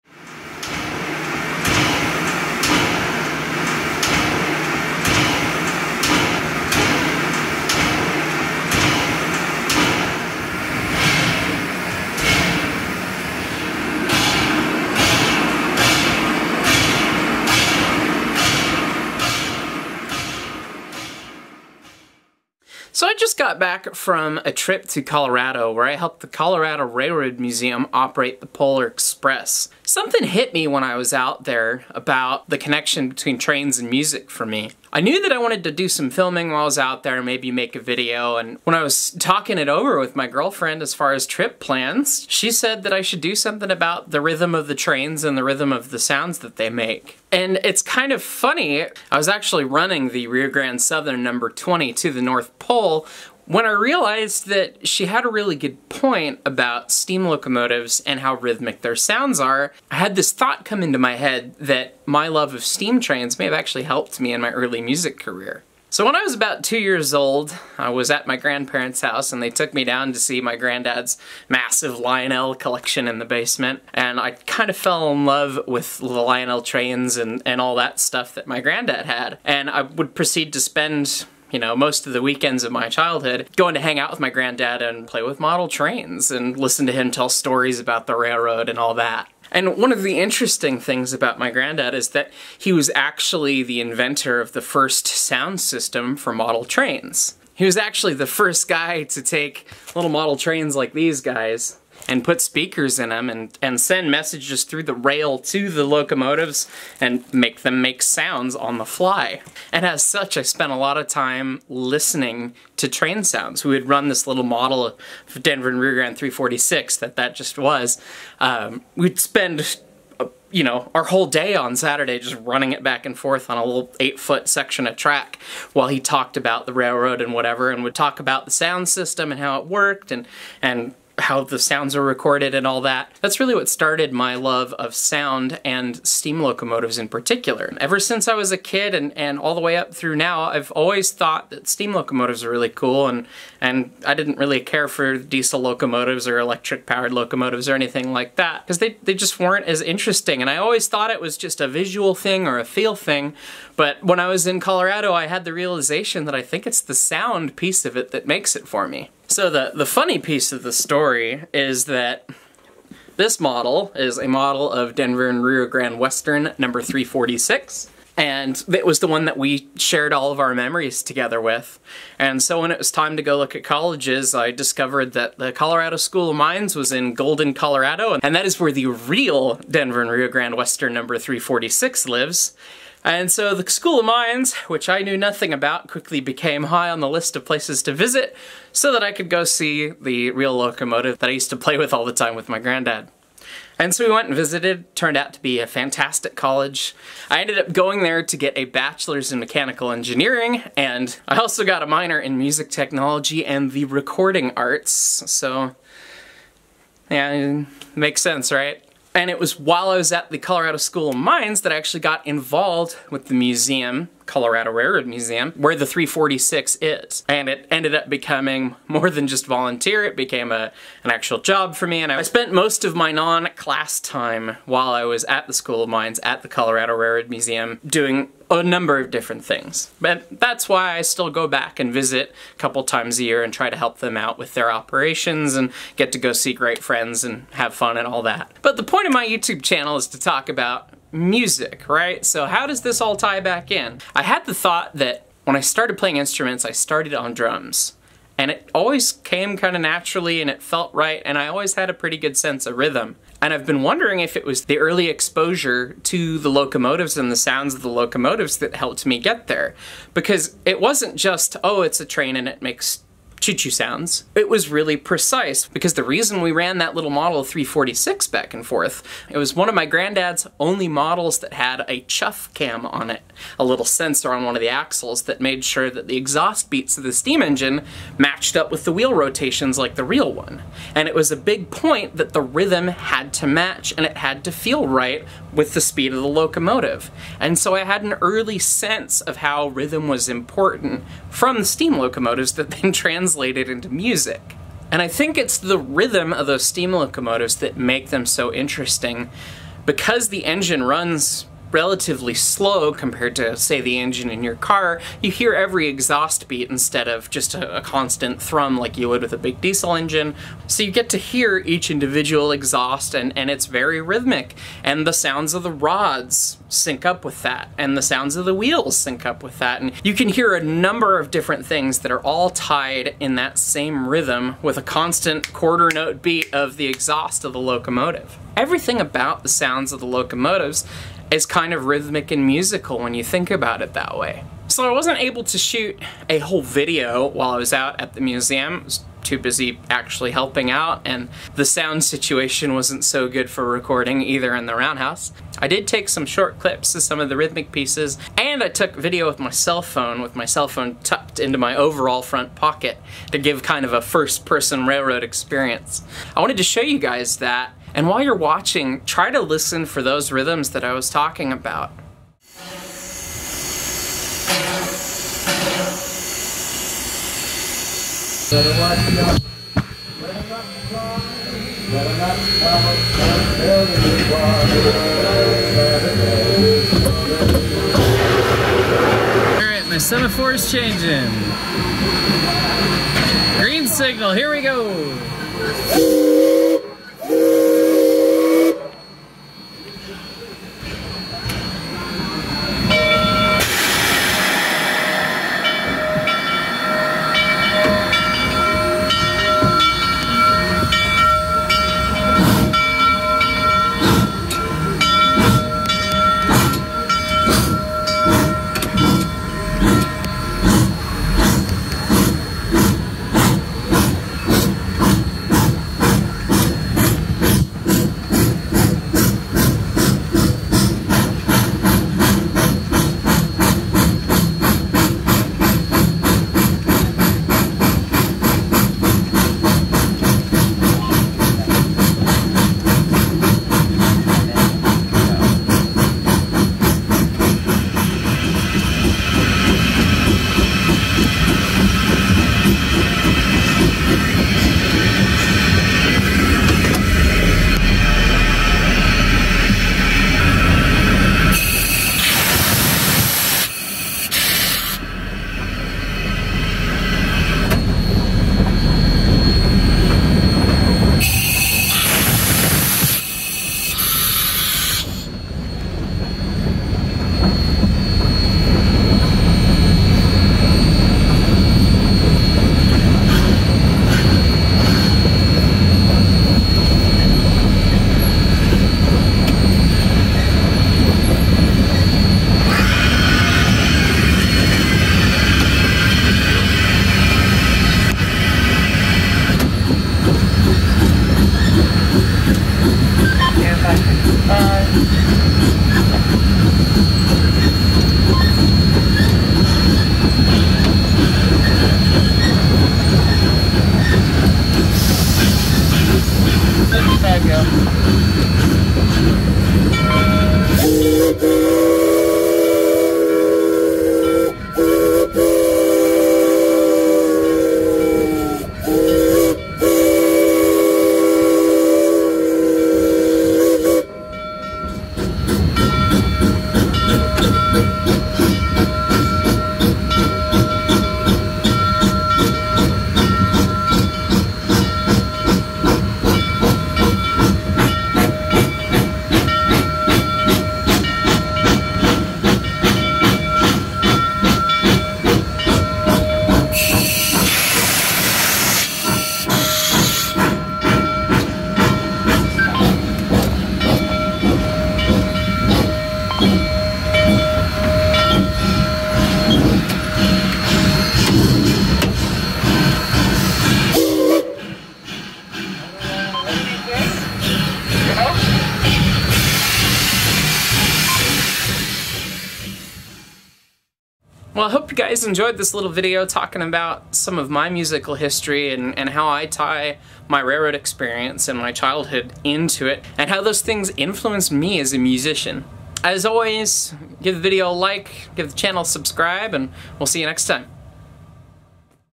How Steam Train Sounds Made a Musician
In this video, I talk about how I grew up listening to steam train sounds and how I realized that they influenced my sense of rhythm. We also get a behind-the-scenes look at running the Rio Grande Southern #20 at the Colorado Railroad Museum for the Polar Express!